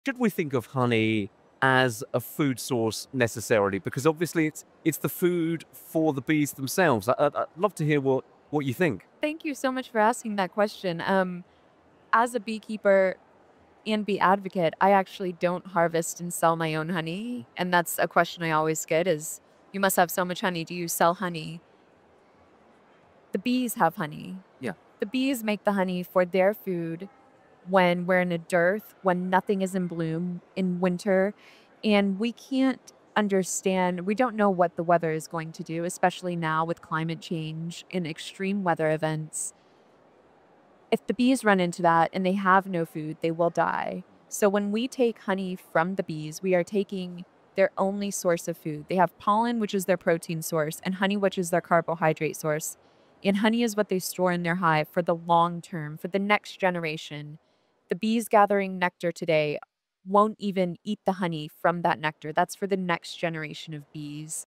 "We are taking their ONLY food source!" Why @TexasBeeworks DOESN'T harvest honey from her bees
“I actually DON’T harvest and sell my own honey.” @TexasBeeworks explains why honey is so important for bees